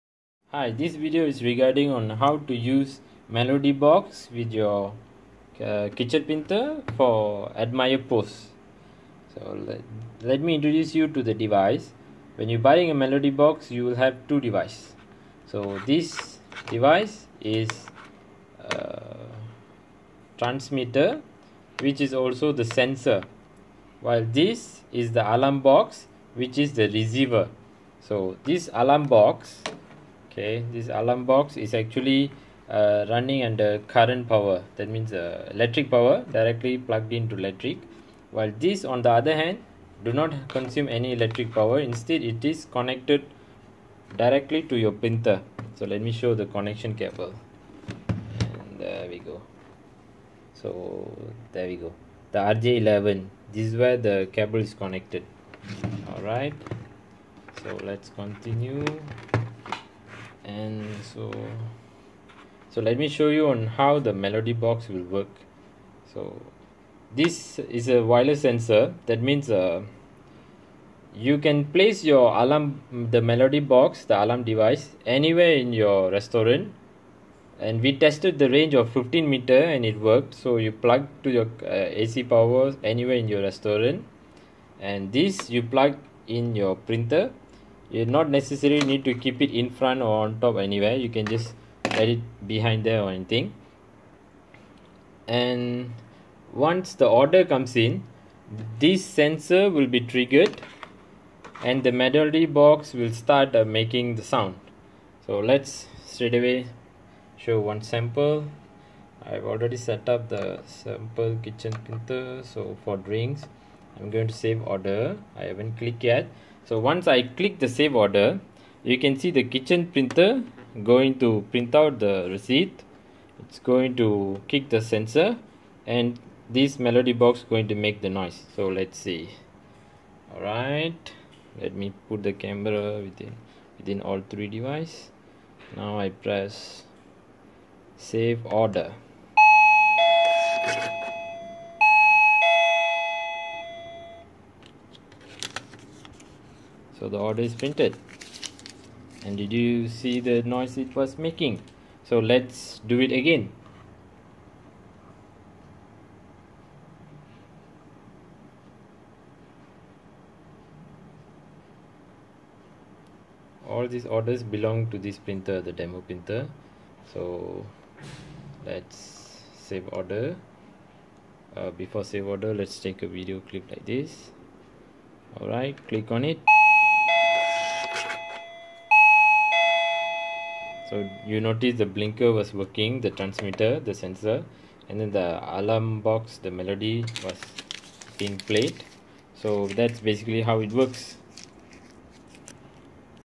Kitchen Thermal Printer Alarm or Melody Box for Admire F&B POS System
In this video we demonstrate on how the Melody Box or Alarm Box works for Admire F&B POS System once its connected to the Kitchen Thermal Printer (or also known as Order Slip Printer).

This Alarm Box can be turned off as it runs on electric power. When your kitchen staff or chef is going to take rest away from the kitchen, then they can switch on the power of this Alarm Box. So when new order comes in, this Alarm Box will produce the loud Alarm music and the Chef can be aware of this new order and return back to Kitchen to cook the new order. Thus this device will be very helpful for the kitchen team.

View our website admirepos.com to learn more about Admire F&B POS System as well as to browse through our other product and packages.